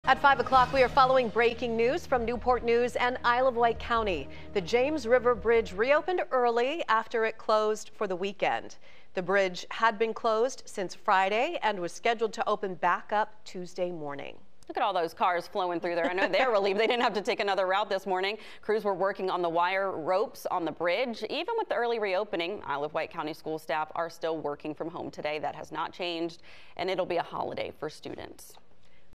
James River Bridge reopens ahead of schedule
The James River Bridge, which was scheduled to be closed from 1 a.m. Friday, Feb. 2 to 5 a.m. Tuesday, Feb. 6 for maintenance work, has reopened one day ahead of schedule on Feb. 5 at approximately 12:10 a.m.